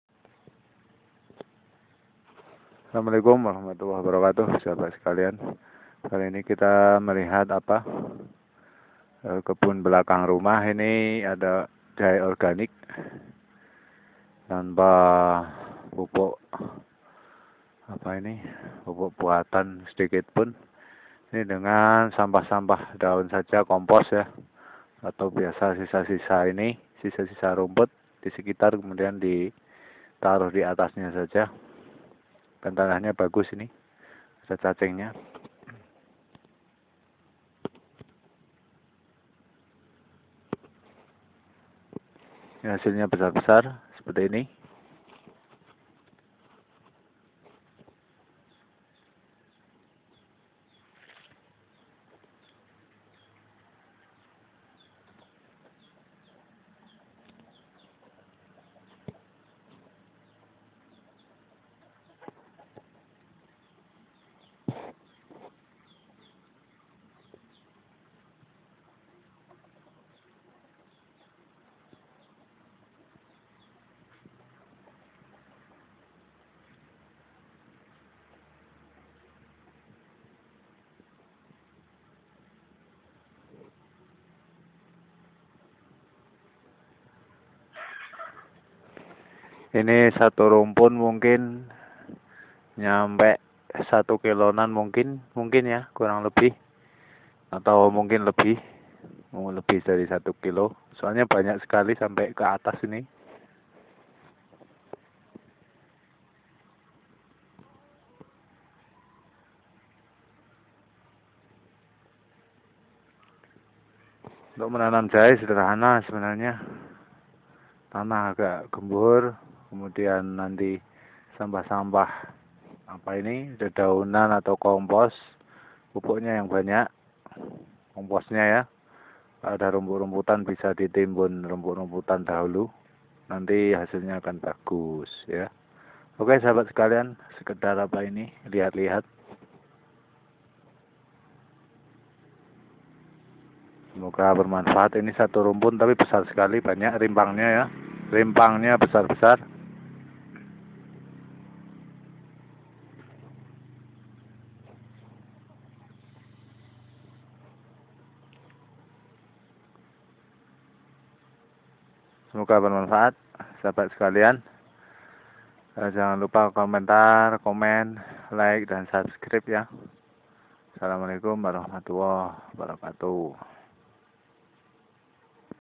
Jahe organik dibelakang rumah
Jahe organik dibelakang. Rumah .. Dengan pupuk cabutan rumput seadanya.